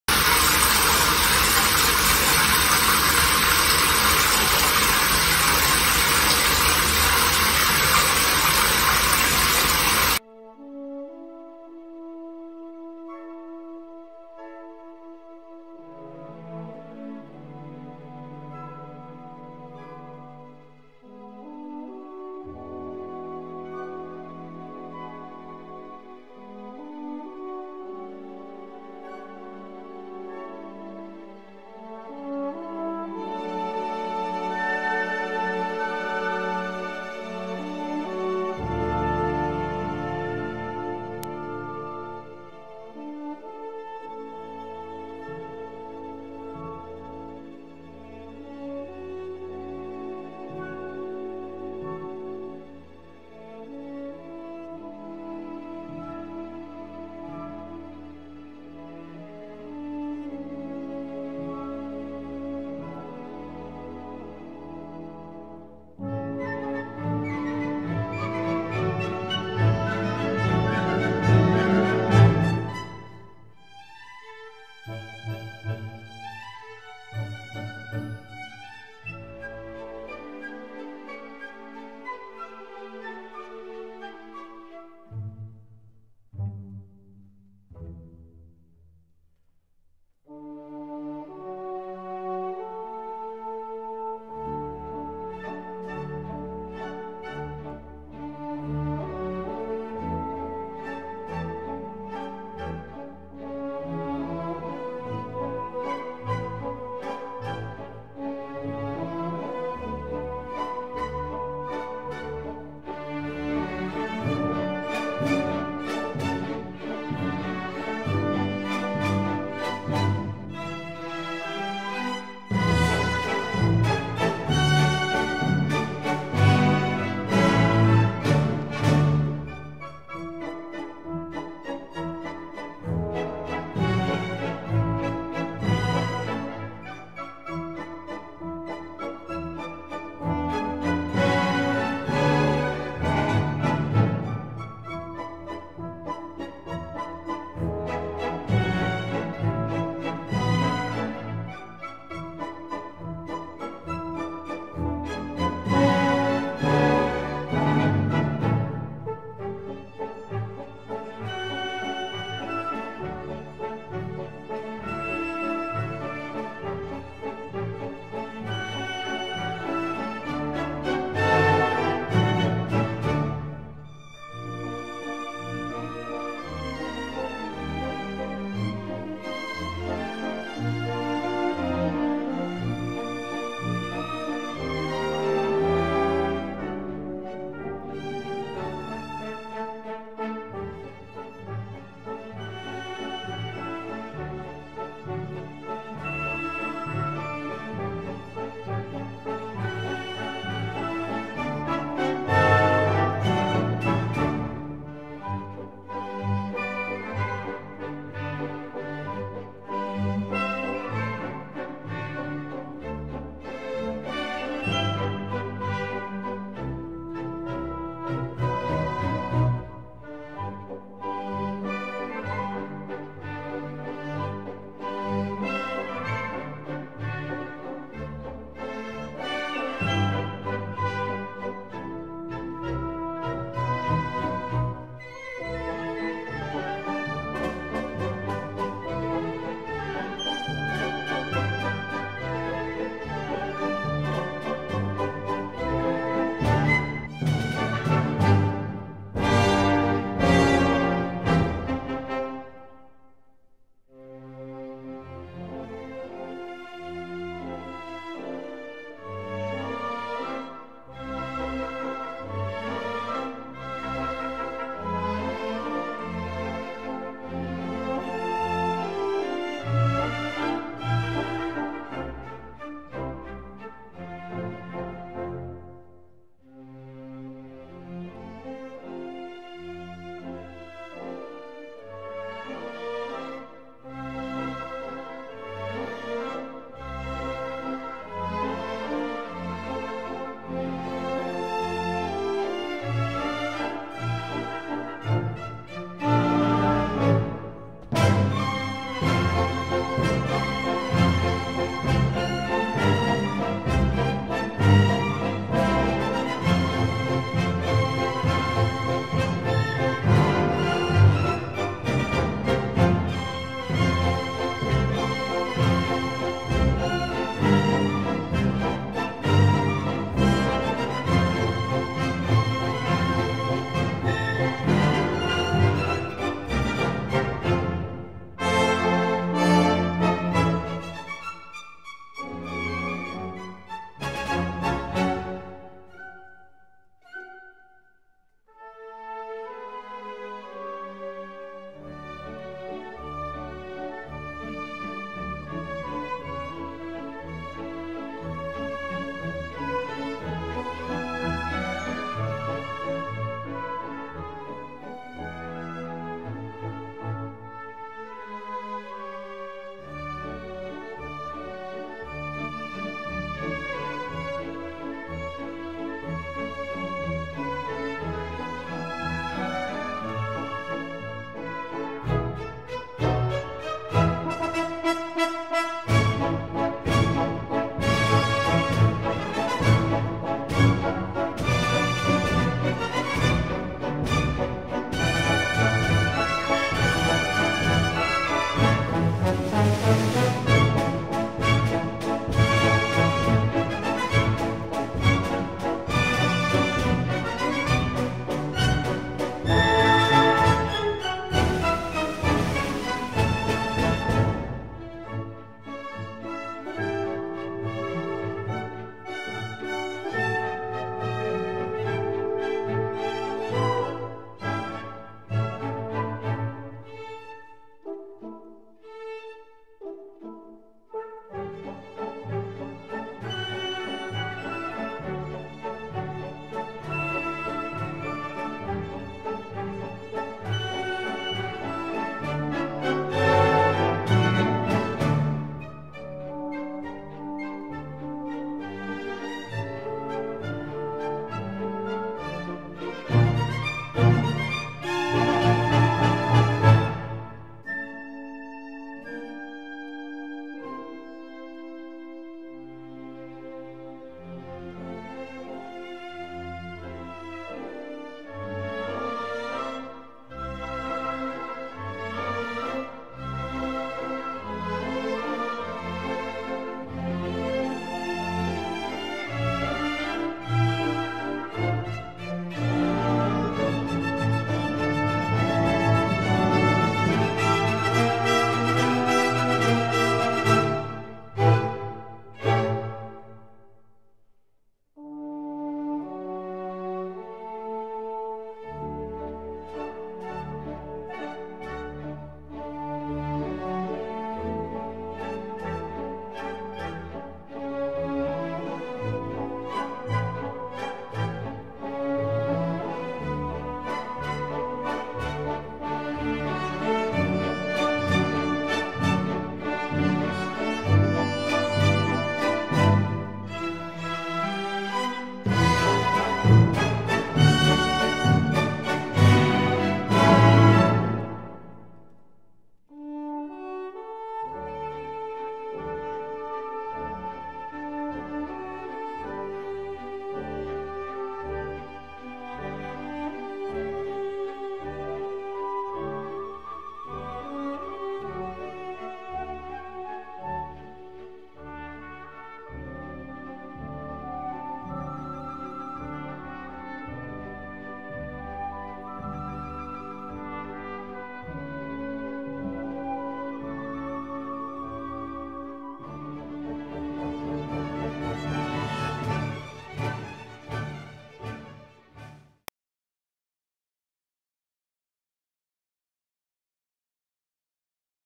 Bay Rum Bar Soap By Kilted Suds | Cold Process Soap Making
Fresh essence of bay leaves with the bright enjoyment of orange peel and zest.
Mica Colorant from Nurture Soap
Lustrous Brown